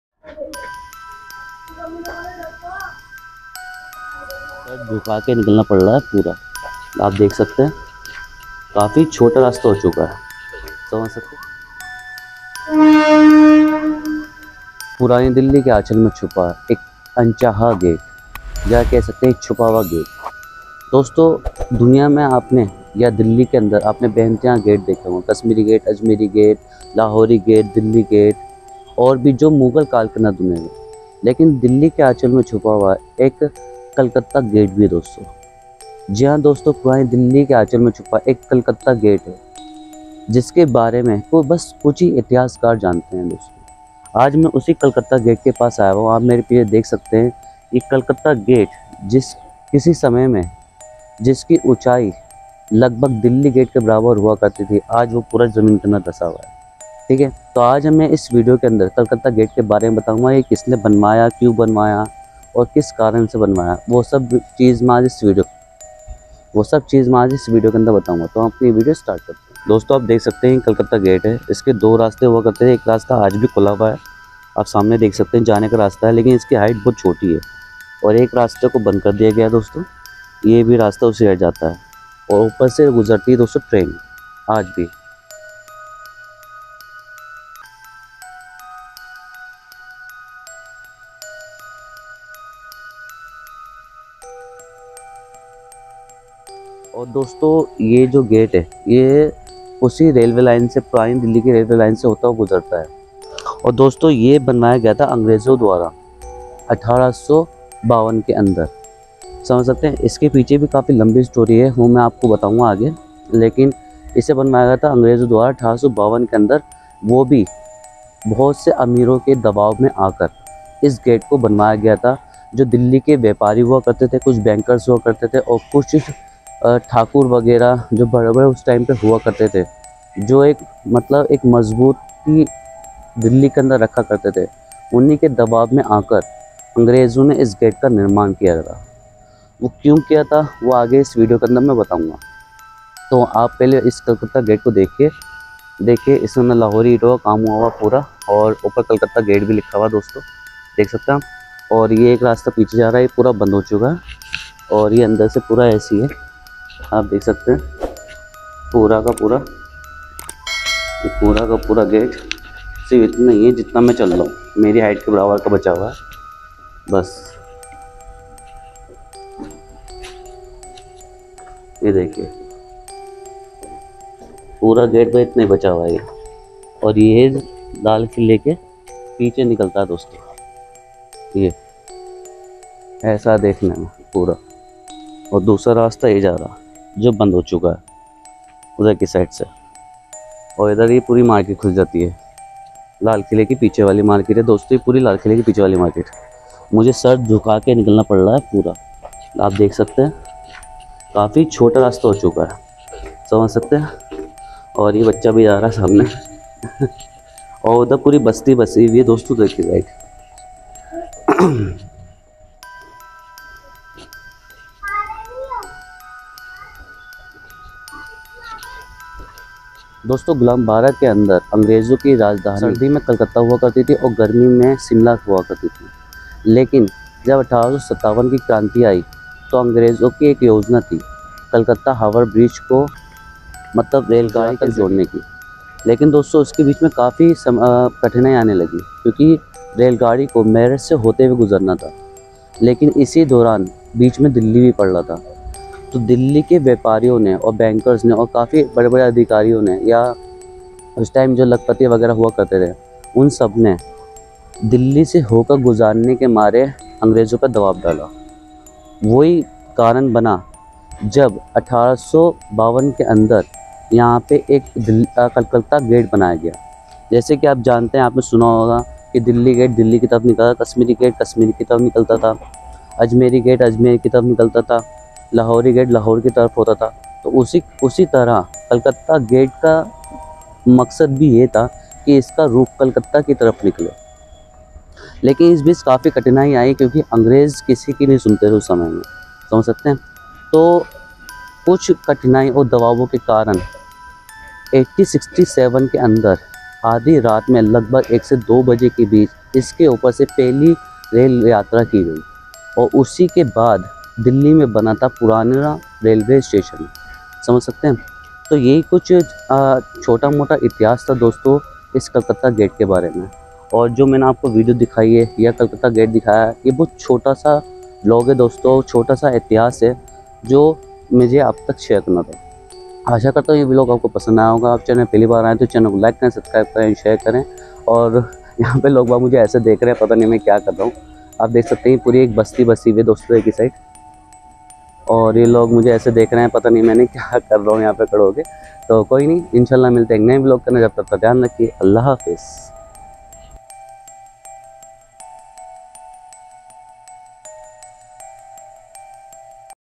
1852 मे यहाँ झुकना पढ़ा अंग्रेजो को Calcutta gate | historical places | heritage sites | old delhi
1852 मे यहाँ झुकना पढ़ा अंग्रेजो को Calcutta gate | historical places | heritage sites | old delhi

दिल्ली में आपने कश्मीरी गेट, लाहौरी गेट, मोरी गेट, अजमेरी गेट व दिल्ली गेट को तो सुना व देखा ही होगा, जिनका निर्माण मुगलकाल में हुआ था। ये सभी मुख्य मार्ग से सटे होने के कारण नजर में आ जाते हैं, लेकिन दिल्ली में एक ऐसा गेट भी है, जिसकी जानकारी शायद ह आपको हो। यह कलकत्ता गेट है, जिसका निर्माण अंग्रेजों ने कराया था।
पुरानी दिल्ली के आंचल में छिपा कलकत्ता गेट लाल किले के पीछे की तरफ स्थित है, जो अपने साथ दिल्ली का भी इतिहास समेटे हुए है। कलकत्ता गेट का निर्माण सन् 1852 में हुआ था। लेखक व इतिहासकार सोहेल हाशमी बताते हैं कि गुलाम भारत में अंग्रेजों की राजधानी गर्मी में शिमला, जबकि सर्दी में कलकत्ता हुआ करती थी। वह बताते हैं कि वर्ष 1857 की क्रांति के बाद अंग्रेजों की योजना कलकत्ता स्थित हावड़ा को रेलगाड़ी के जरिये पंजाब से जोड़ने की थी। रेलगाड़ी को मेरठ होते हुए जाना था, लेकिन अंग्रेज दिल्ली से रेलवे लाइन गुजारने के लिए तैयार नहीं थे। ऐसे में पुरानी दिल्ली के व्यापारी, बैंकर्स व कुछ अमीरों ने इकट्ठा होकर अंग्रेजों पर दिल्ली से रेलवे लाइन गुजारने के लिए दबाव डाला। व्यापारियों व अन्य लोगों ने अंग्रेज अधिकारियों को समझाया था कि दिल्ली व्यापारिक दृष्टि से काफी महत्वपूर्ण है। ऐसे में इसे रेल लाइन से जोड़ा जाना चाहिए। इसके लिए व्यापारियों ने कई याचिकाएं भी डाली थीं, जिसके बाद ईस्ट इंडिया कंपनी के बोर्ड ऑफ कंट्रोल के अध्यक्ष चा‌र्ल्स वुड ने रेल लाइन को दिल्ली से गुजारने के लिए हामी भरी थी। इसके बाद वर्ष 1867 में 1 जनवरी की आधी रात में दिल्ली में रेल इसी गेट के ऊपर से होकर गुजरी थी और उसी समय के आसपास पुरानी दिल्ली रेलवे स्टेशन की नींव पड़ी थी।
इतिहासकार ने बताया कि जिस तरह से दिल्ली में कश्मीरी गेट कश्मीर जाने के लिए था, लाहौरी गेट लाहौर की ओर निकला था, संभवत: उसी तरह कलकत्ता गेट से कलकत्ता की ओर जाने वाला रास्ता हुआ करता था। क्योंकि, कलकत्ता को बसाने वाले अंग्रेज ही थे और सन् 1852 में कलकत्ता पहुंचने के लिए सड़क के अलावा और कोई माध्यम नहीं था।